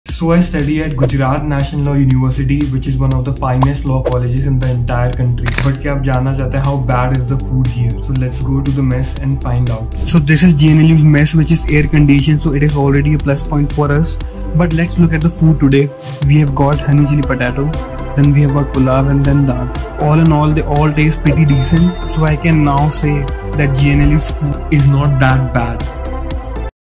How is Food at an NLU?
I have been studying at GNLU for a year and this is my take on the food being served at the mess.